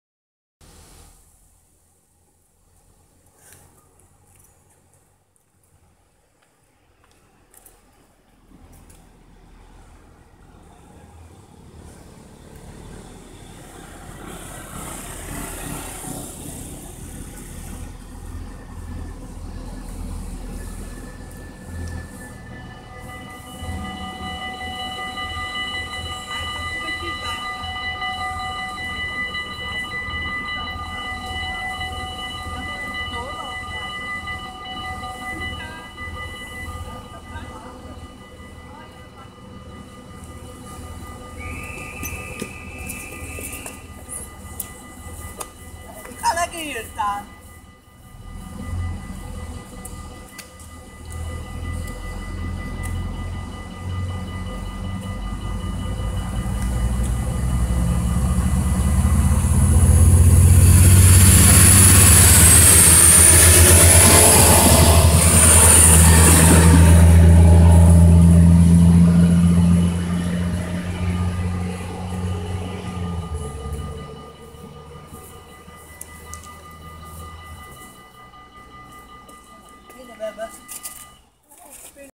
Spoorwegovergang Hengelo Gezondheidspark
Passeren: LINT 41/H als ST Zutphen-Oldenzaal
Traject: Hengelo-Zutphen/Elementis
Locatie: Station Hengelo Gezondheidspark

Deze video is gemaakt op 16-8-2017 samen met Spoorwegovergangen Duitsland en OV in Nederland